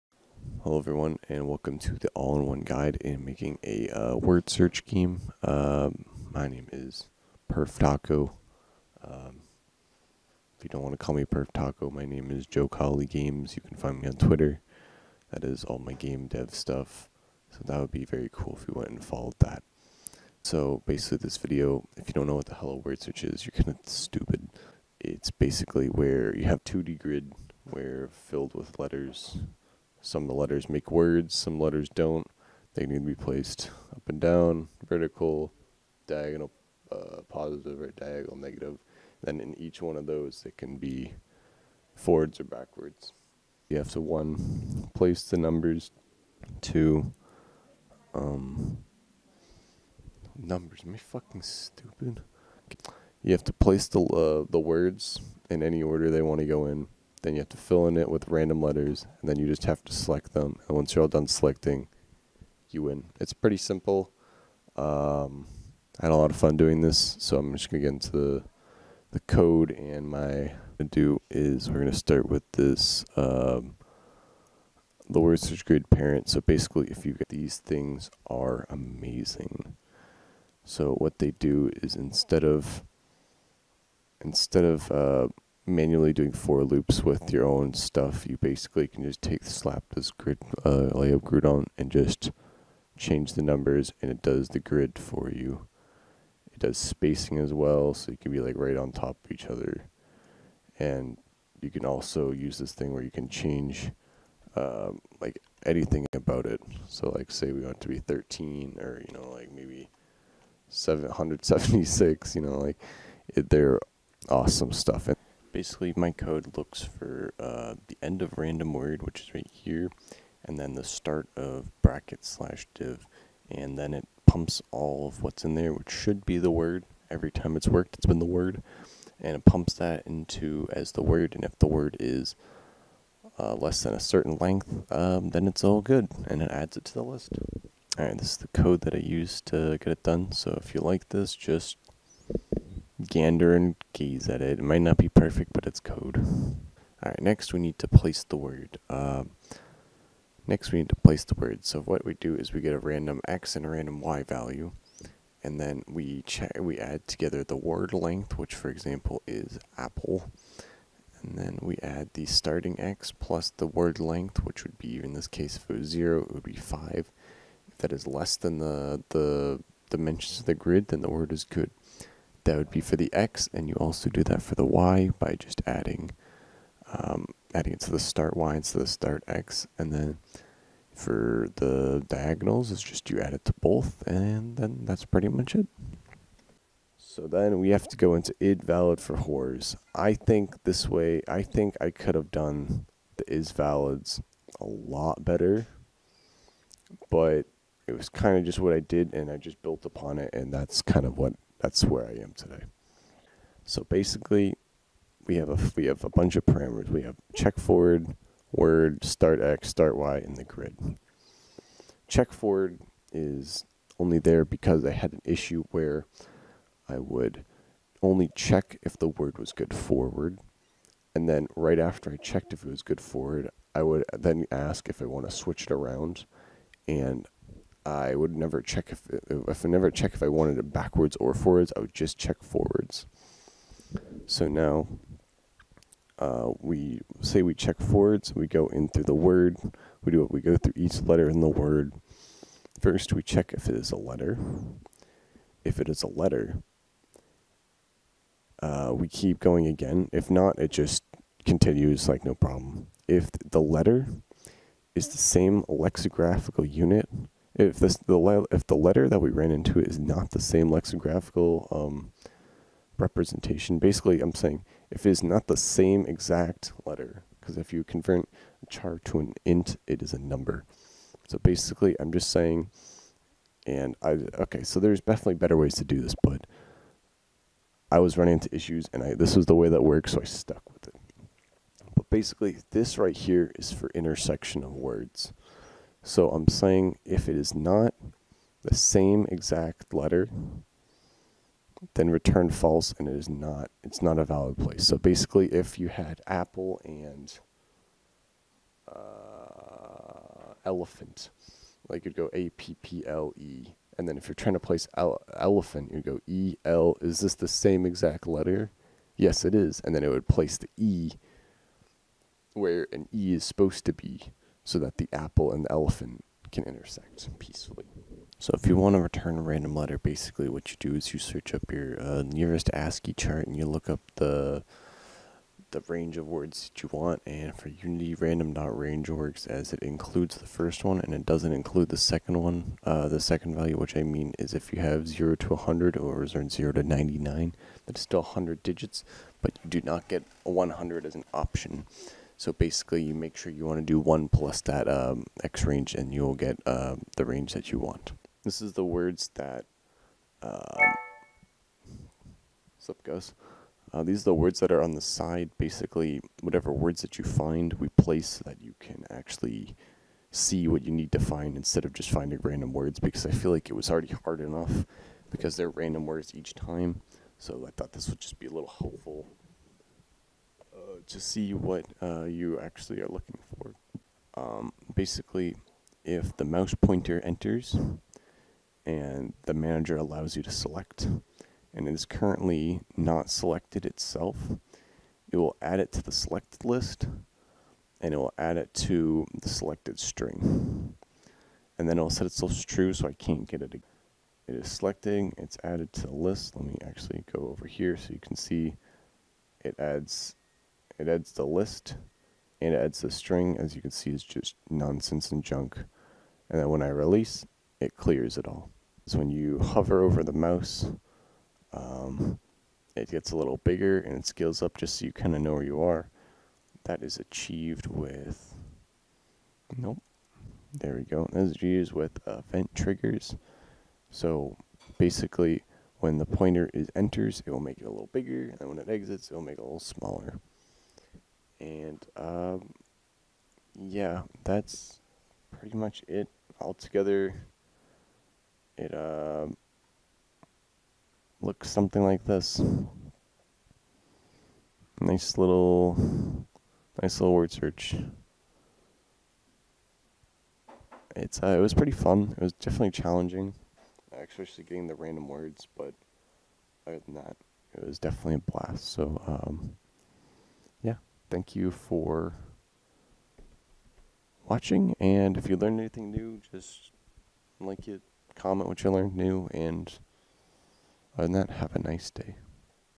Making an Random Word Search Generator in UNITY!
Am i alive?
yes

sad and semester ended

Content???
My microphone is dead, I used a rock band on in this video
Getting one for chirstmas, then ill be back hugs and kisses

Here you go David

~PerfTaco